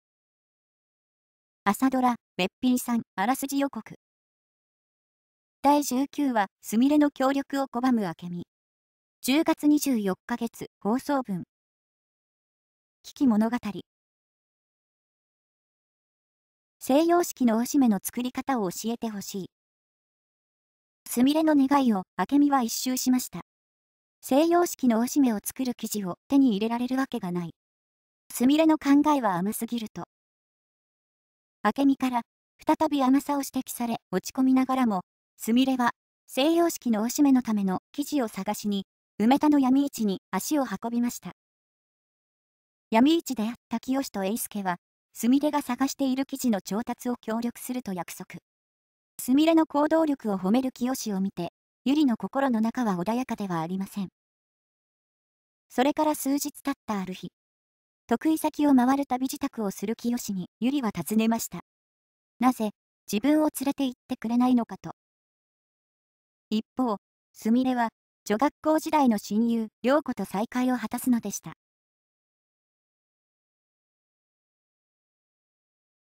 朝ドラ「べっぴんさん」あらすじ予告 第19話 10月24日（月）放送分 －聴きものがたり
朝ドラ「べっぴんさん」あらすじ予告 第19話 すみれの協力を拒む明美 10月24日（月）放送分 －聴きものがたり